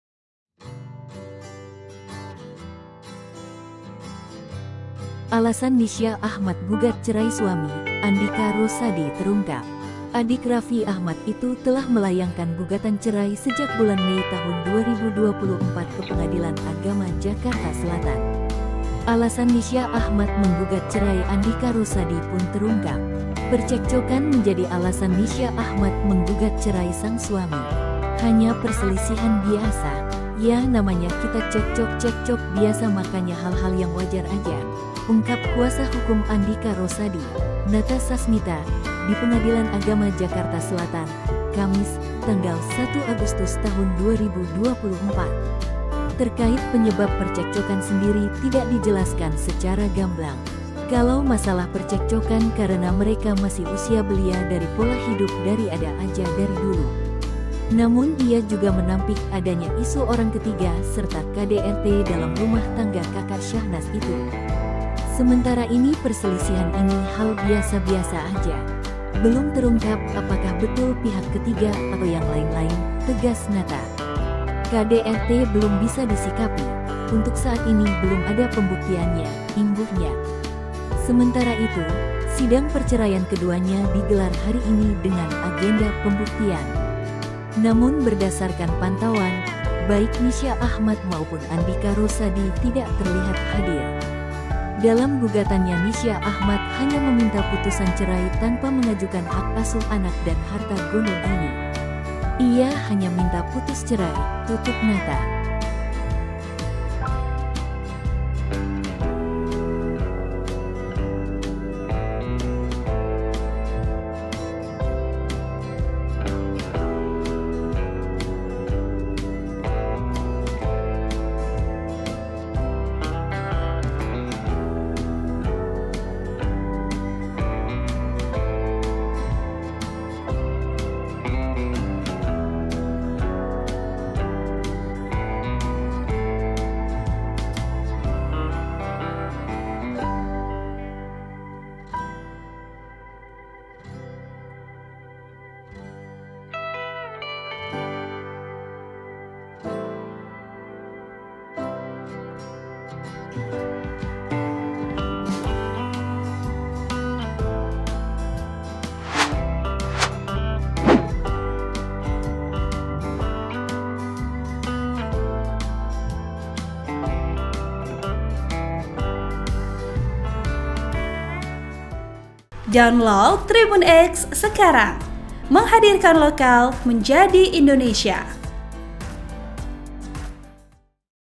Fakta Soal Kabar Nisya Ahmad Ceraikan Andika Rosadi
Alasan Nisya Ahmad gugat cerai suami, Andika Rosadi terungkap.
Adik Raffi Ahmad itu telah melayangkan gugatan cerai sejak bulan Mei 2024 ke Pengadilan Agama Jakarta Selatan.
Alasan Nisya Ahmad menggugat cerai Andika Rosadi pun terungkap.

Percekcokan menjadi alasan Nisya Ahmad menggugat cerai sang suami.
"Hanya perselisihan biasa. Ya namanya kita cekcok-cekcok biasa makanya hal hal yg wajar aja," ungkap kuasa hukum Andika Rosadi, Nata Sasmita, di Pengadilan Agama Jakarta Selatan, Kamis (1/8/2024).

Terkait penyebab percekcokan sendiri tidak dijelaskan secara gamblang.
"Kalau masalah percekcokan karena mereka masih usia belia dari pola hidup dari ada aja dari dulu.

Namun ia juga menampik adanya isu orang ketiga serta KDRT dalam rumah tangga kakak Syahnaz itu.
"Sementara ini perselisihan ini hal biasa-biasa aja. Belum terungkap apakah betul pihak ketiga atau yang lain-lain," tegas Nata.
"KDRT belum bisa disikapi, untuk saat ini belum ada pembuktiannya," imbuhnya.

Sementara itu, sidang perceraian keduanya digelar hari ini dengan agenda pembuktian.
Namun berdasarkan pantauan, baik Nisya Ahmad maupun Andika Rosadi tidak terlihat hadir.
Dalam gugatannya Nisya Ahmad hanya meminta putusan cerai tanpa mengajukan hak asuh anak dan harta gono gini.
"Iya hanya minta putus cerai," tutup Nata.

Program: -
Host: -
Editor Video: Haris
Uploader: -